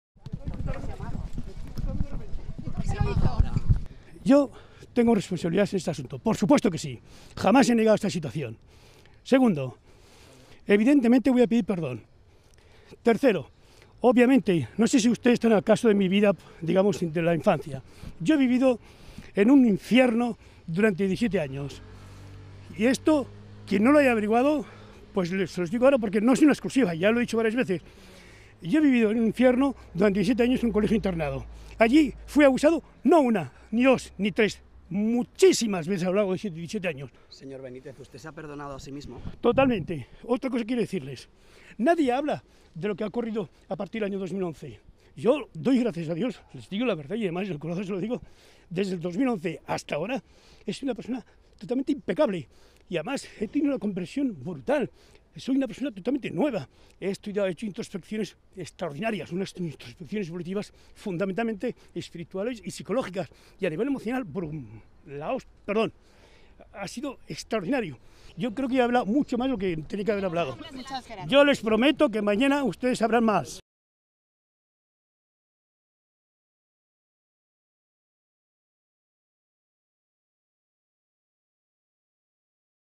JOAQUÍN BENÍTEZ pide perdón a sus VÍCTIMAS
Cortesía: EL PAIS

El ex profesor del colegio de los Maristas de Les Corts pide, ante la prensa, perdón a sus víctimas.

¡SÍ LE GUSTA LA VERDAD COMPARTELO!